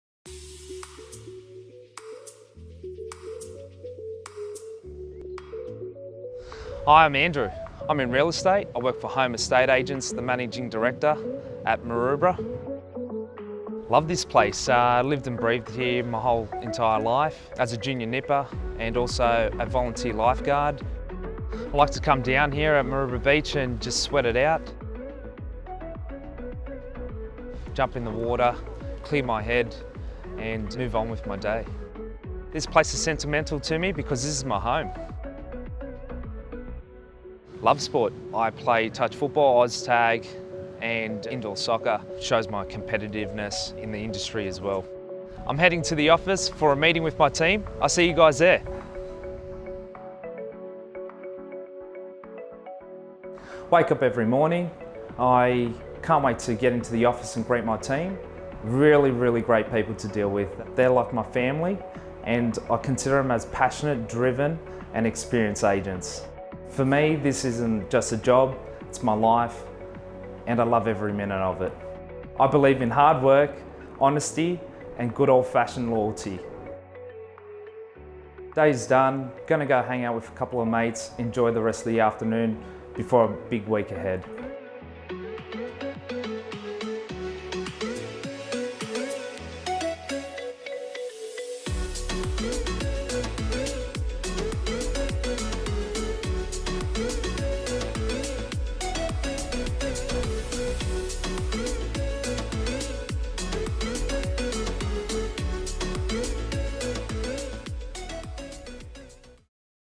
Agent Profile Video Series: Andrew Anastassiou
“This isn’t just a job for me, It’s my life and I love every minute of it!” Andrew Anastassiou, Director of home estate agents Maroubra.

As part of our agent profile series, Andrew shares with us his personal side, what motivates him and how he stays grounded in such an ultra competitive industry. He also shares with us a glimpse into his professional life with his team at the Maroubra home estate agents office.